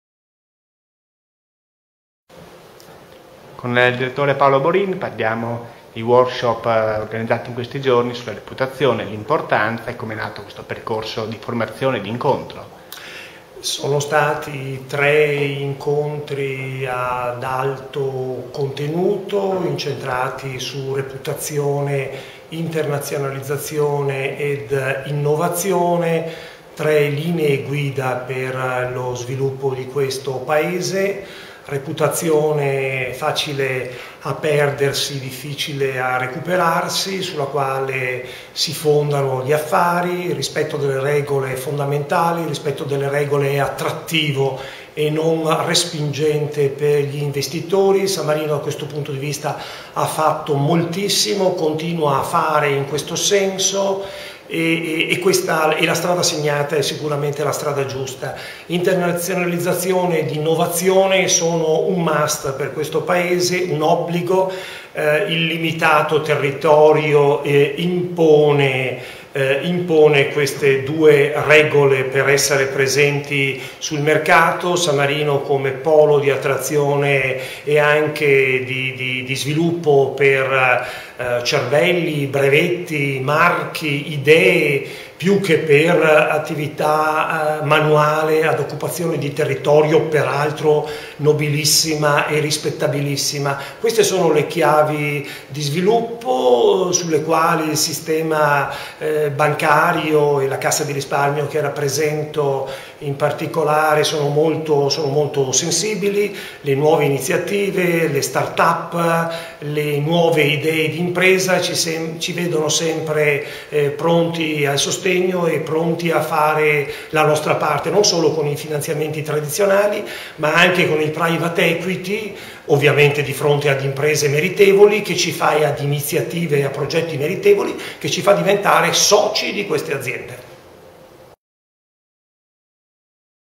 Paolo Borin (Carisp RSM): la reputazione
Reputazione, internazionalizzazione ed innovazione. Il Direttore Generale Cassa di Risparmio della Repubblica di San Marino Paolo Borin, ai tre temi oggetto dei workshop organizzati dalla Fondazione San Marino, aggiunge "la comunicazione. Il ciclo di incontri è stato ad alto contenuto. L'internazionalizzazione e l'innovazione devono essere un must per il Paese: San Marino deve diventare un polo di attrazione e sviluppo per cervelli, brevetti, marchi e idee".